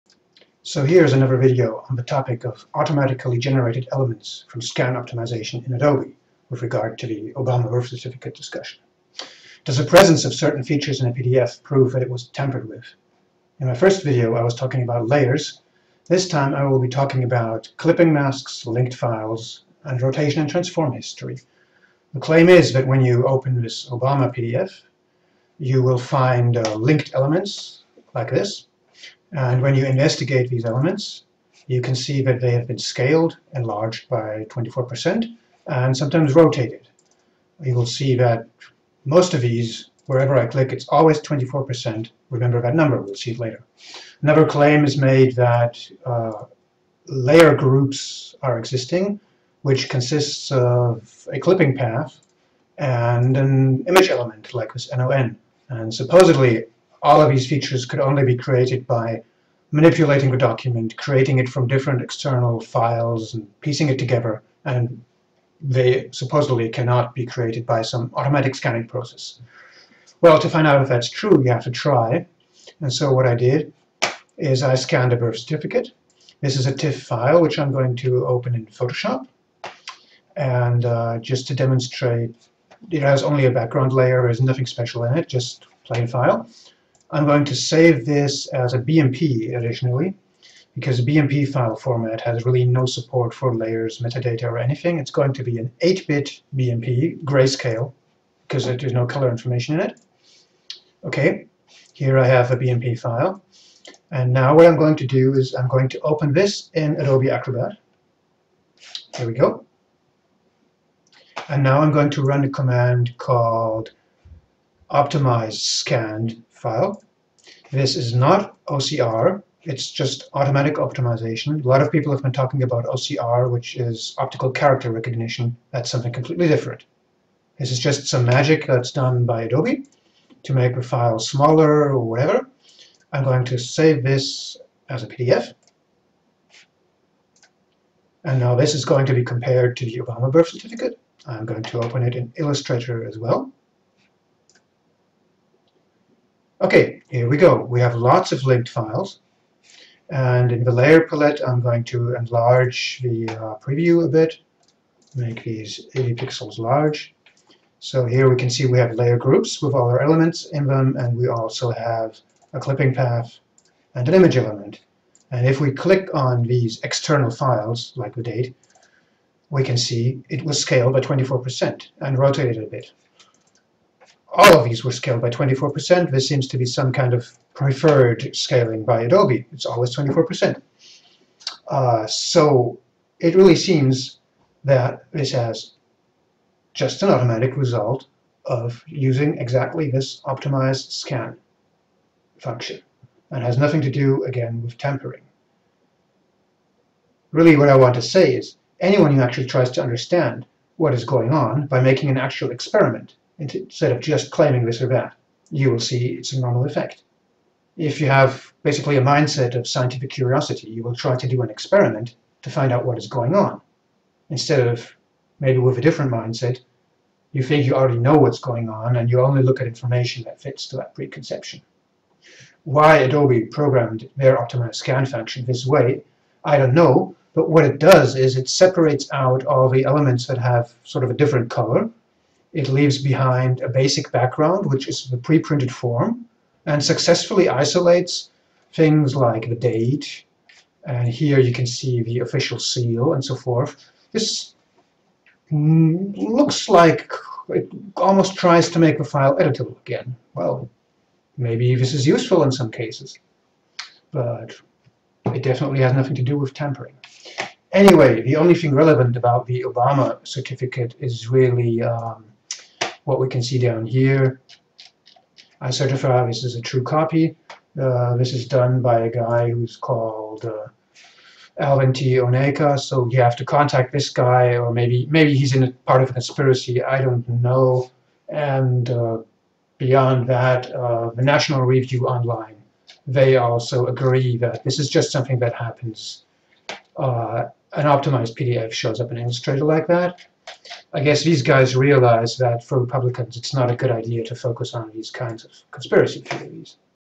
Obama PDF Explained - Linked elements, transform + rotation history, clipping masks
Explanation why rotation history, linked elements etc are expected to appear in a scanned file that was put through "optimize scan"

So here is another video on the topic of automatically generated elements from scan optimization in Adobe

Does the presence of certain features in a PDF prove that it was tampered?

In my first video I was talking about layers.

This time I will be talking about clipping masks, linked files, and rotation and transform history.

The claim is that when you open the Obama certificate in Illustrator like this,

you can find elements in the "Links" palette that have a rotation and transform history

When you click on them you can see that it says this was scaled by 24% and rotated a bit.

What's interesting that many of these elements are scaled by 24% exactly.

also in the layer palettes you will find there are groups of layers that always consist of an image and a clipping mask.

Supposedly the presence of these elements „proves" that the file must have been forged or manipulated.

It is claimed that it is impossible that such elements could be innocently created by some automatic processing in Adobe.

I wanted to find out for myself.

Now here I have a sample birth certificate I scanned on April 27. It's a plain TIFF with no layers, here are the metadata, with the scan date, in a plain text editor.

I will save this in BMP format because BMP really can't store any special information.

Now I will open that in Acrobat and use the "Optimize Scan function"

Note - this has nothing to do with OCR.

I will save the resulting file as a PDF and open in illustrator.

See how in the links palettes, all the interesting stuff like stamps and dates and signatures appears to be an external object.

And what happens when I click on them - there it is, rotation and transform history, and again you have those 24 % all over the place.
I don't get any elements with 90! rotation but that's probably just because of which way you scan the paper, portrait or landscape.

In the layers palette you can see groups of layers with images and clipping masks.

So you can see, anyone who actually tries to understand what is going on, by making an actual experiment, instead of just claiming this or that - you will see that this is a normal effect of the optimize scan function.

If you have a mindset of scientific curiosity, you will try to do an experiment to find out what is going on.

There is a different mindset,  that already thinks it knows what's going on from the beginning, and only looks at any information that will fit into that preconception.

Why optimize Scan was  programmed that way by Adobe, is another question.

Otherwise, the only thing relevant about the Obama PDF is this anyway: "I certify this is a true copy  bla bla Alvin T Onaka"

Otherwise, if anyone wanted to forge something, they would forge the paper document, not a PDF.

See the National Review Online for further explanation from a  US conservative/Republican viewpoint.

They agree that this is just how an optimized PDF of a scan shows up in Illustrator.

And they understand that it's probably not too smart for Republicans to get caught up in these kinds of conspiracy theories.